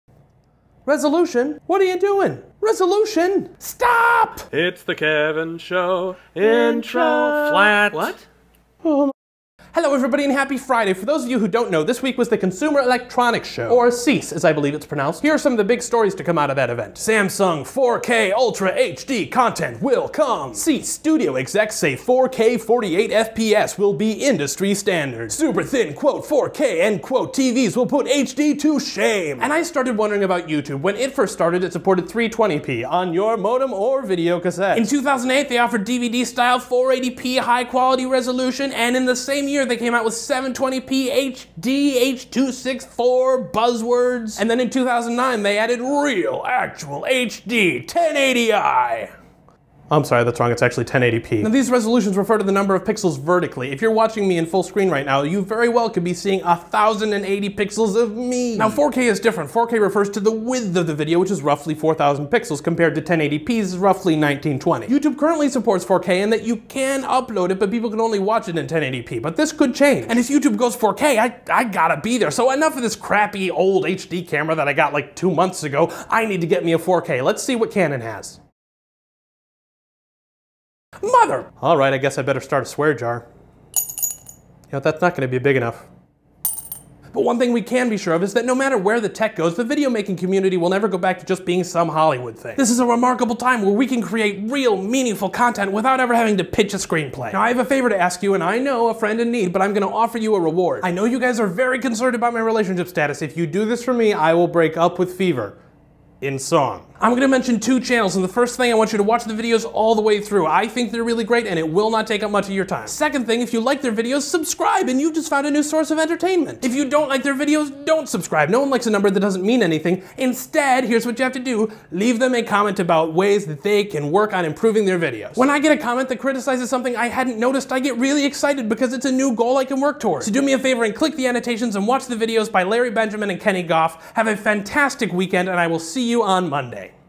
4096 Resolutions!
Happy Friday, everyone. Today, a bit of a chat about the biggest thing to come out of the Consumer Electronic Show: 4K videos.

Intro courtesy of Tom McNichols